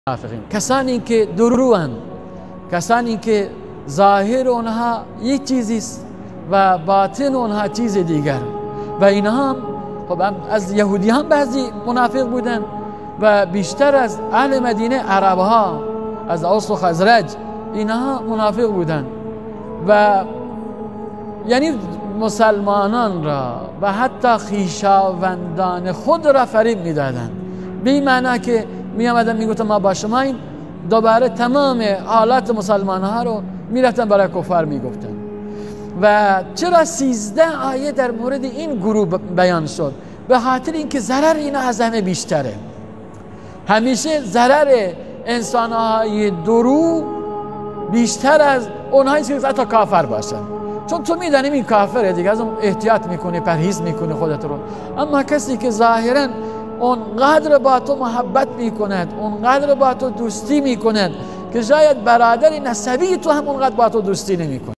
صحبت های مولانا نعمت الله کاشانی در مورد انسان هایی همچون مولوی عبدالحمید که دو رو هستند
صحبت های مولانا نعمت الله کاشانی در مورد انسان هایی همچون مولوی عبدالحمید که دو رو هستند و همیشه در خدمت رسانی به کفار در صف اول قرار دارند